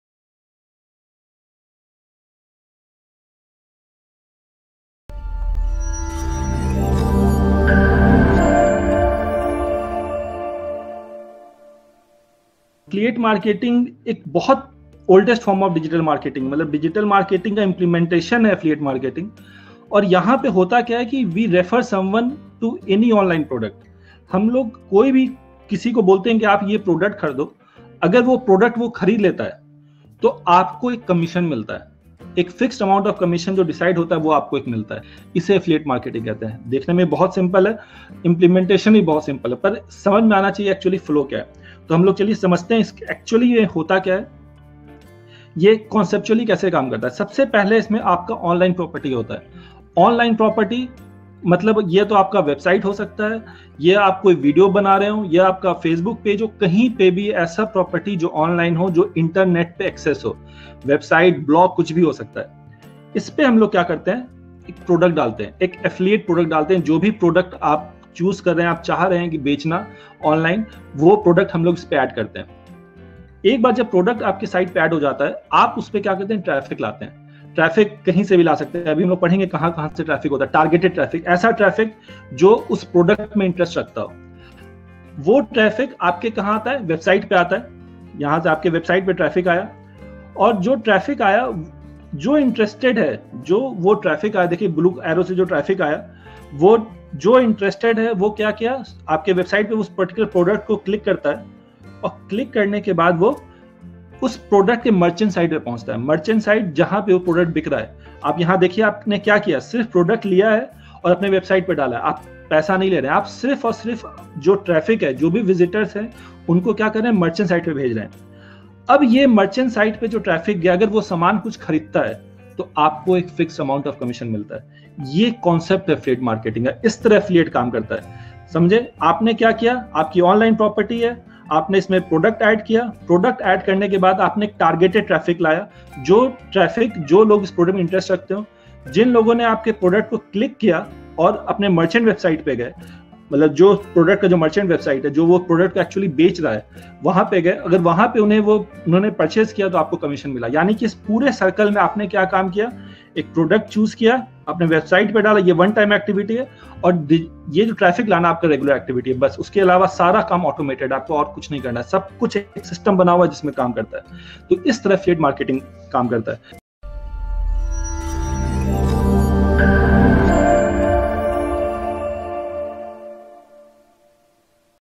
What is affiliate marketing ? Glimpse of Live Webinar by Semsols Tech Pvt Ltd.
Register for Free Webinar's

This presenter of this webinar is Piyush Jain, having 15 years of experience in Digital marketing. Affiliate marketing is his core niche.

He regularly conducts webinar on many topics relating to digital marketing & its implementation to earn online.

Register to this link & you will be informed about the next upcoming free webinar.

Topic Covered in this webinar
Learn affiliate marketing & Earn Online From Home

How much money you can make in affiliate marketing industry?
What you should know to get in affiliate marketing business?

Do you need any Paid tools to start affiliate marketing ?